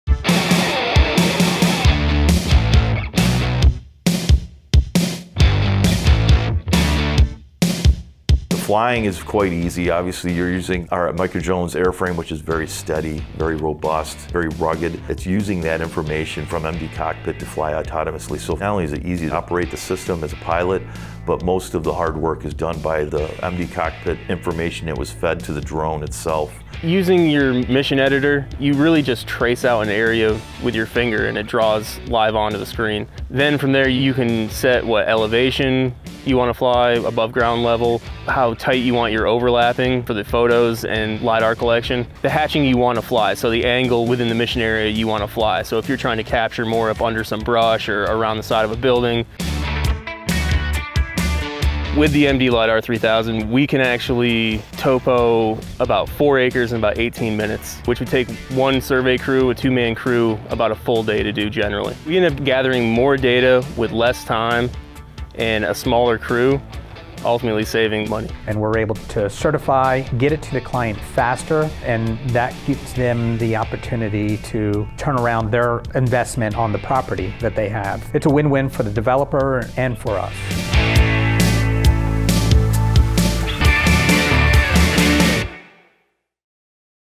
Microdrones mdLiDAR3000 and ESP - Plan, FLY, Process and Visualize
Microdrones mdLiDAR3000 is getting put to good use by the folks at ESP Associates as documented in our latest series of videos demonstrating the Microdrones intuitive workflow of Plan, Fly, Process & Visualize.